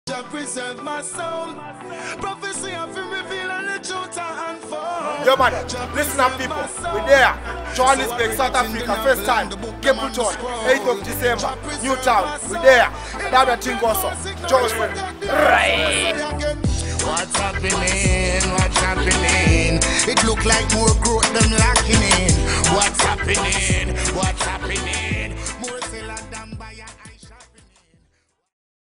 08 Dec 2012 Johannesburg Newtown Comes ALIVE 08 Dec 2012 Capleton King of Fire! ARE YOU READY?
This December ushers in what promises to be, one of the most phenomenal Reggae experiences to ever hit South African soil: Headliner - Capleton King of Fire! 08 Dec 2012 - Newtown Park, Johannesburg.

LIONESS PRODUCTIONS 2012